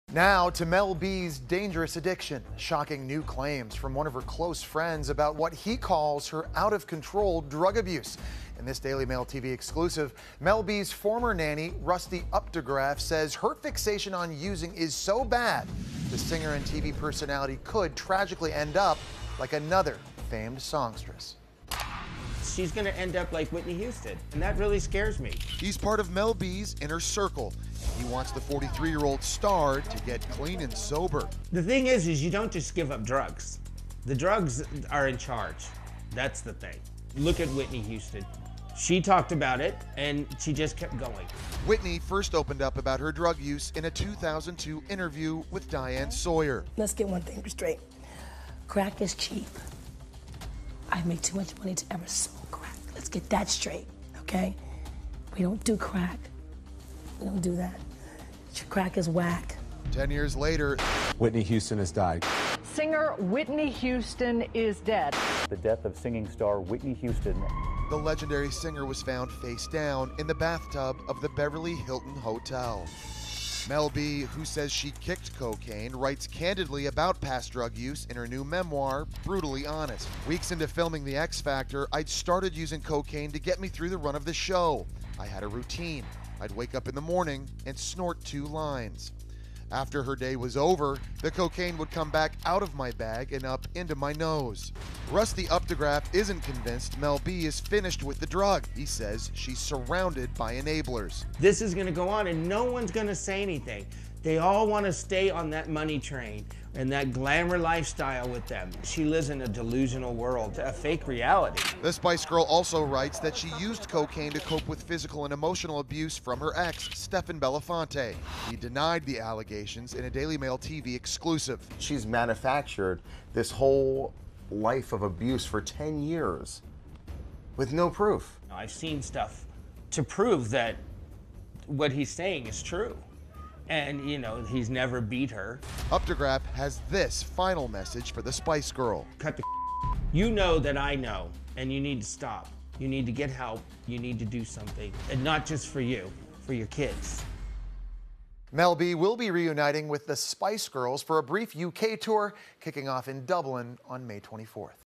Mel B's ex-nanny speaks out on her drug addiction
Mel B's former nanny Rusty Updegraff, 60, has told DailyMailTV he's worried she'll have the same fate as Whitney Houston after learning of the former Spice Girl's drug use in her new book. Updegraff served as Mel's nanny for nine months, caring for her daughters (pictured left) Angel, 11, (by Eddie Murphy) Madison, seven, (by ex-husband Stephen Belafonte) and Phoenix Chi Gulzar, her 19-year-old daughter by first husband Jimmy Gulzar. The mother of three revealed in her memoir that she struggled with an alcohol and drug addiction but said she has overcome it after moving on from her divorce from ex Stephen Belafonte. However, Rusty claims she did not seek professional help to become sober. The 60-year-old's relationship with the America's Got Talent judge remains strained after he claimed she was a danger to her kids in a court declaration earlier this year.